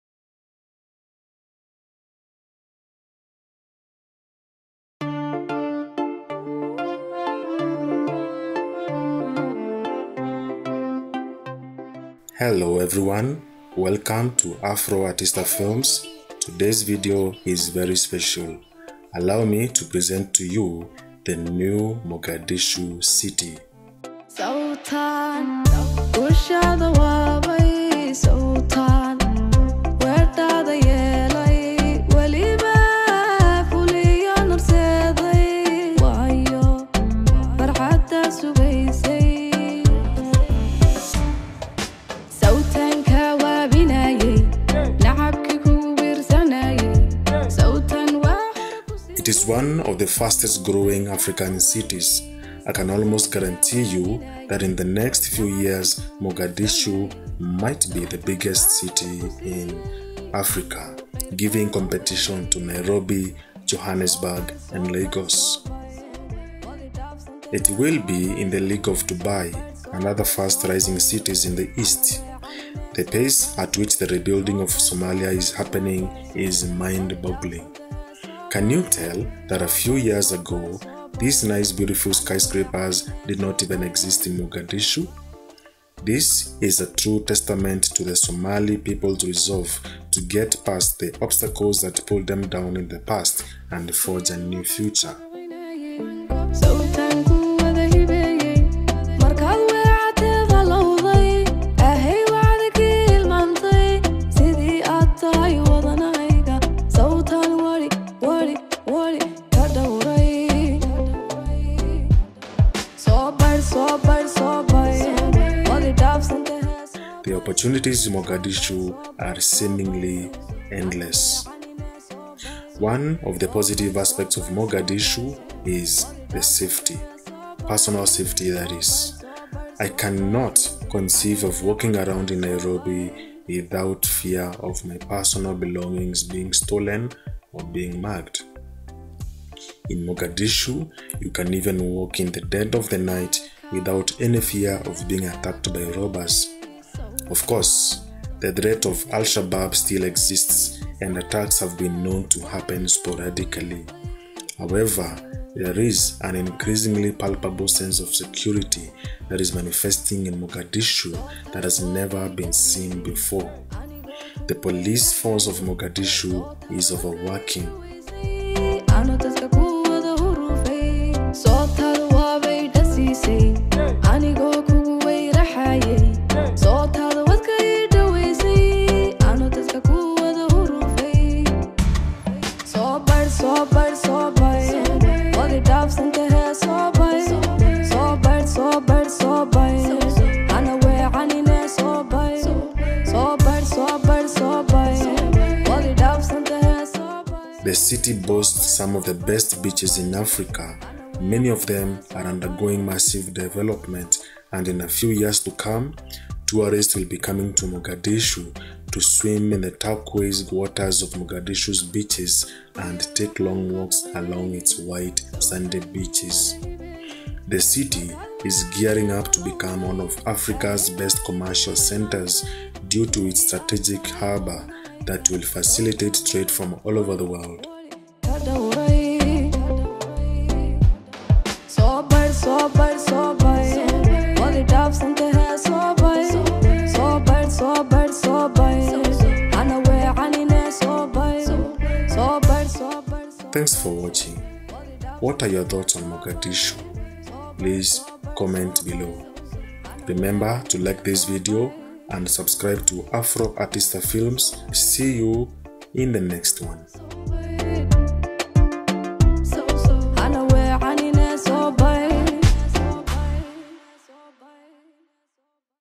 MOGADISHU CITY HAS UNBELIEVABLY CHANGED. (Shocked Kenyan)
Mogadishu city. Allow me to present to you the new Mogadishu City. When other Africans hear about Mogadishu, all they can conjure inside their minds are images of desolate buildings, gun-toting militias, and chaos. In fact, for many Kenyans like me, the thought of going to Mogadishu sends shivers down the back. What if I told you that all of that was a lie? Maybe the fear was justified in the past. But as we speak, the new Mogadishu City is a sight to behold.
 It is one of the fastest growing African cities. I can almost guarantee you that in the next few years, Mogadishu might be the biggest city in Africa, giving competition to Nairobi, Johannesburg, and Lagos. It will be in the league of Dubai and other fast-rising cities in the east. The pace at which the rebuilding of Somalia is happening is mind-boggling. Can you tell that a few years ago, these nice, beautiful skyscrapers did not even exist in Mogadishu? This is a true testament to the Somali people's resolve to get past the obstacles that pulled them down in the past and forge a new future.
You might not know it, but allow me to break it to you. Mogadishu is one of the places with a lot of opportunities for Africans to work and live. Right now, as we speak, there are thousands of Kenyans who have already established themselves in Mogadishu, especially in the education and hospitality industries. and they leave without fear. and I'm not just talking about Kenyan Somalis.
If things go the way they are going, Eastleigh in Nairobi might relocate to Mogadishu. The opportunities in Mogadishu are seemingly limitless. of the positive aspects of Mogadishu is safety. personal safety, that is. I cannot conceive of walking around in Nairobi without fear of my personal belongings being stolen or being mugged. In Mogadishu, you can even walk in the dead of the night without any fear of being attacked by robbers. Of course, the threat of al-Shabaab exists and attacks have been known to happen sporadically. However, there is an increasingly palpable sense of security that is manifesting in Mogadishu that has never been seen before. The police force of Mogadishu is overworked. One very good reason for this sense of security is, of course, Islam. Islam is the religion of Somalia and it dictates social norms and behavior. Being a very religious country, theft is heavily frowned upon.
In the hospitality industry of Mogadishu, there is immense potential for growth. The city boasts some of the best beaches in Africa. Many of them are undergoing massive development. In a few years to come, tourists will be coming to Mogadishu to swim in the turquoise waters of Mogadishu's beaches and take long walks along its white sandy beaches.
The city is gearing up to become one of Africa's best commercial centers due to its strategic harbour that will facilitate trade from all over the world. Indeed, Mogadishu will be the new Dubai of Africa.